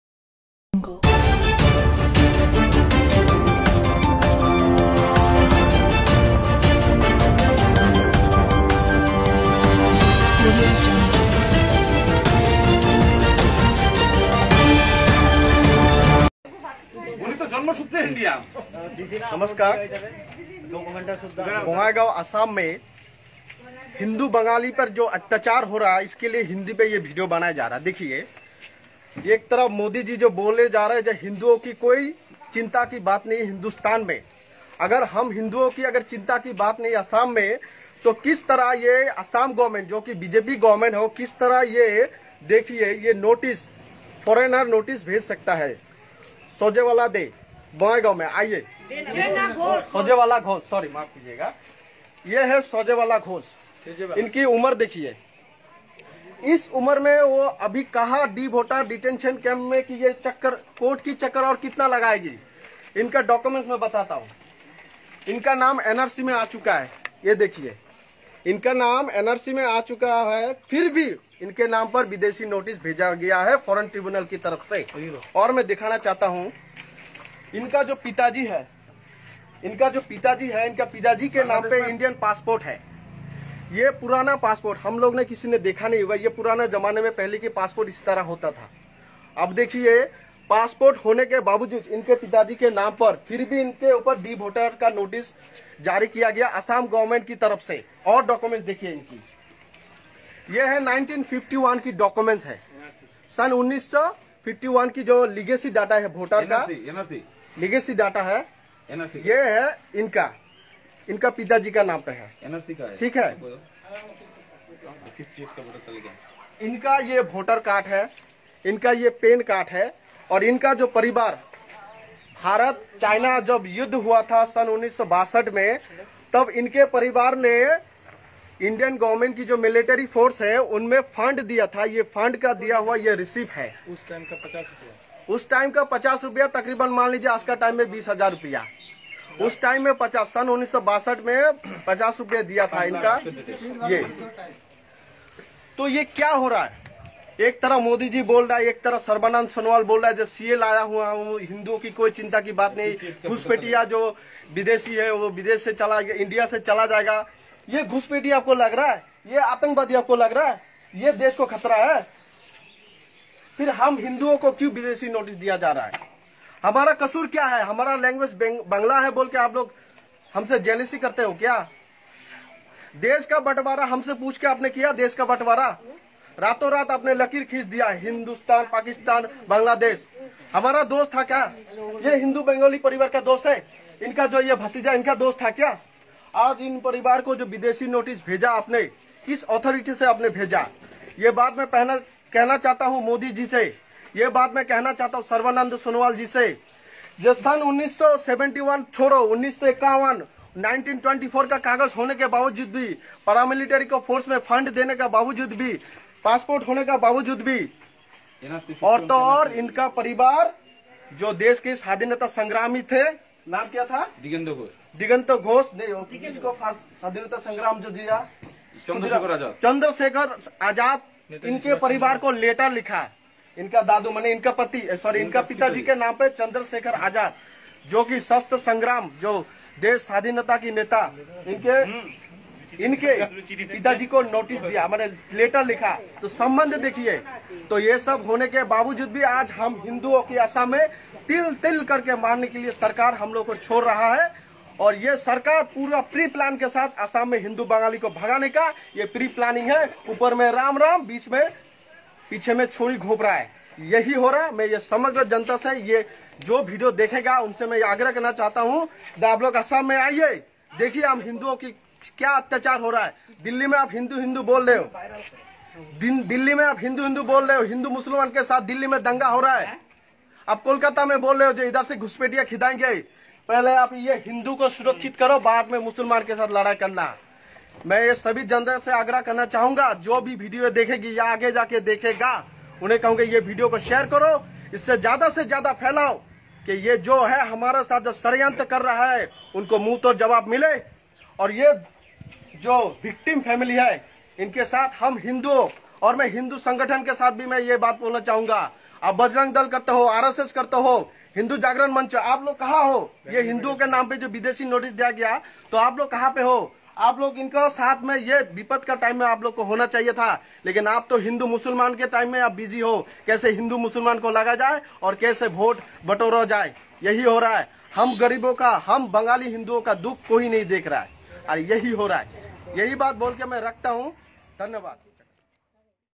ASAM MAI MUSLIM PAR HI NAHI HINDUS PAR HORA HA HAI ZULM BJP PARTY KA .
Ye video mai ek 70 saal ke grandmother KO  bina waje court ke chakkar Katna par raha hai NRC mai naam ane par bhi notice bhej rahi hai bjp government .